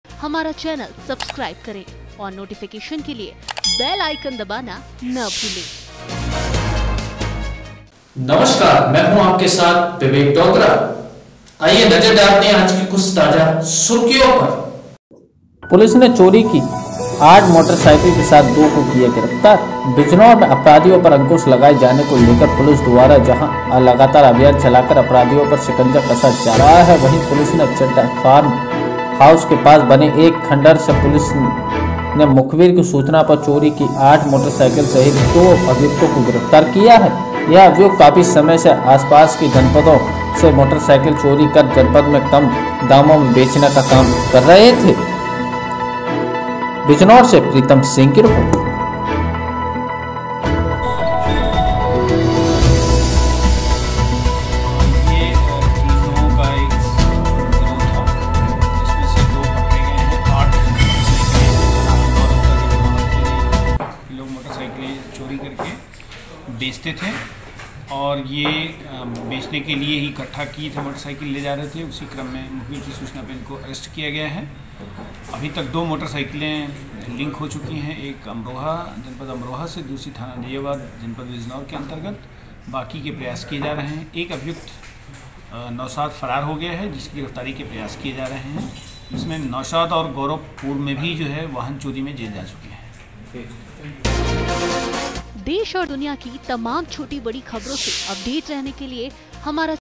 *पुलिस ने चोरी की 8 मोटरसाइकिल के साथ दो को किया गिरफ्तार*
ब्यूरो रिपोर्ट प्रीतम सिंह*
     *जिला  बिजनौर*

       *बिजनौर से*
         *बड़ी खबर*

*आज दिनांक* 7/02/2020को
*बिजनौर।अपराधियों पर अंकुश लगाए जाने को लेकर पुलिस द्वारा जहां लगातार अभियान चलाकर अपराधियों पर शिकंजा कसा जा रहा है। वहीं पुलिस ने चड्ढा फार्म हाउस के पास बने एक खंडहर से पुलिस ने मुखबिर की सूचना पर चोरी की 8 मोटरसाइकिल सहित दो अभियुक्तों को गिरफ्तार किया है। यह अभियुक्त काफी समय से आसपास के जनपदों से मोटरसाइकिल चोरी कर जनपद में कम दामों में बेचने का काम कर रहे थे।*

*एसपी सिटी लक्ष्मी निवास मिश्र ने बताया कि स्वाट टीम व पुलिस टीम को मुखबिर से सूचना मिली थी कि हल्दौर थाना क्षेत्र के चड्ढा फॉर्म हाउस के पास चोरों द्वारा चोरी की गई मोटरसाइकिल को बेचा जा रहा है। मौके पर पहुंची पुलिस ने घटनास्थल से दो अभियुक्त गौरव और रोहित को गिरफ्तार किया है। जबकि इनका तीसरा साथी नौशाद पुलिस को चकमा देकर फरार होने में कामयाब हो गया है।एसपी सिटी ने बताया कि गौरव रोहित और नौशाद काफी समय से आसपास जनपद सहित जिले में मोटरसाइकिल को चुराकर कम दामों में लोगों को बेचने का काम कर रहे थे। साथ ही यह कम दामों में मोटरसाइकिल को बेचकर रुपया कमाने का काम कर रहे थे। पुलिस ने आज इन्हें गिरफ्तार कर लिया है और इन्हें जेल भेज रही है। इनके ऊपर बिजनौर के हलदौर थाना सहित कोतवाली शहर व अन्य थानों में मुकदमा पहले से पंजीकृत है।*

*बाईट :-लक्ष्मी निवास मिश्र।एसपी सिटी।बिजनौर*